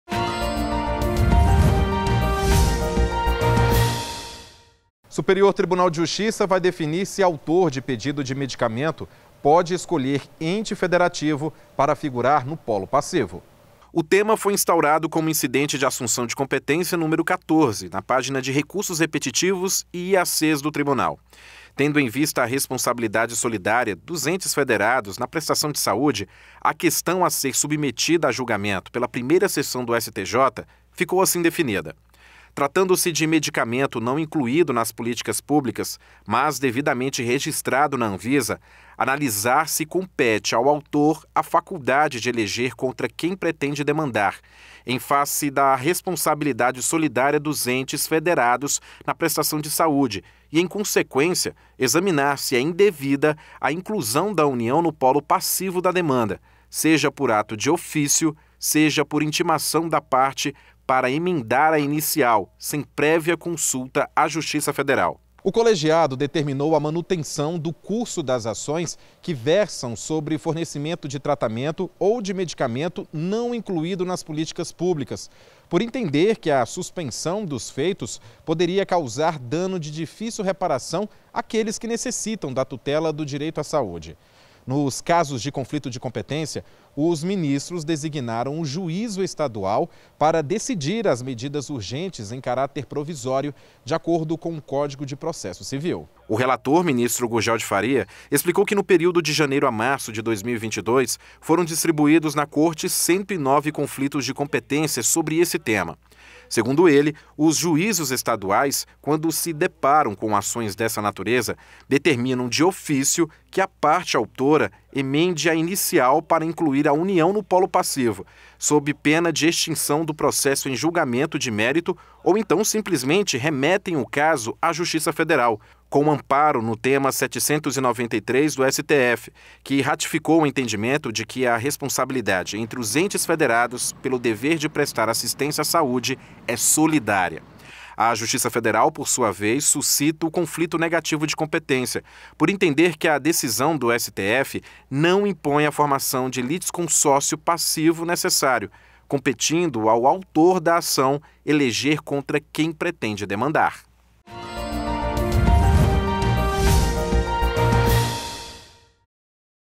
IAC vai definir se autor de pedido de medicamento pode escolher ente para figurar no polo passivo
A Primeira Seção do Superior Tribunal de Justiça (STJ) instaurou Incidente de Assunção de Competência (IAC) para analisar se, tendo em vista a responsabilidade solidária dos entes federados na prestação de saúde, o autor pode escolher contra qual deles mover a ação para fornecimento de medicamento não incluído em políticas públicas, mas devidamente registrado na Anvisa – o que pode afetar a competência para o julgamento da causa.
CC 187276
CC 187533
CC 188002